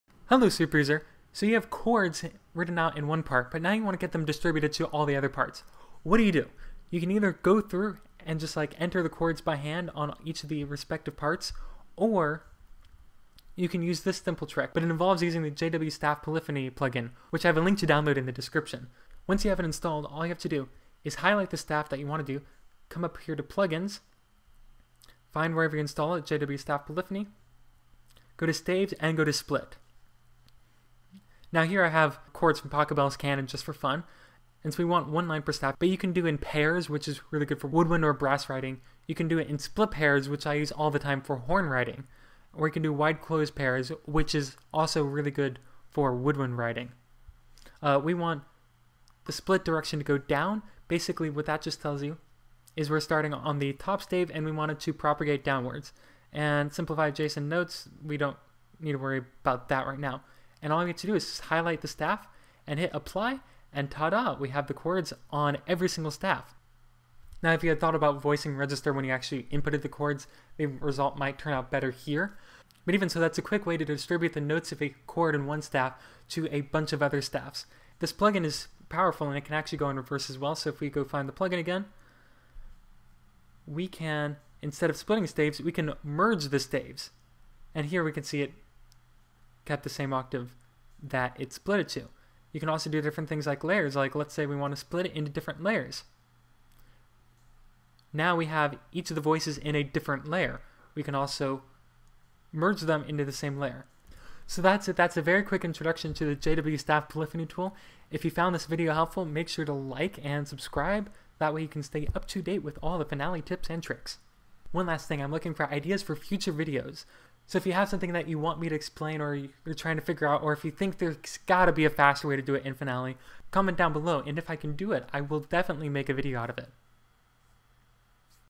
Expand Chords | Finale Superuser
This is how to expand a chord on one staff to several other staves in Finale 25. It uses the JW Staff Polyphony plug-in.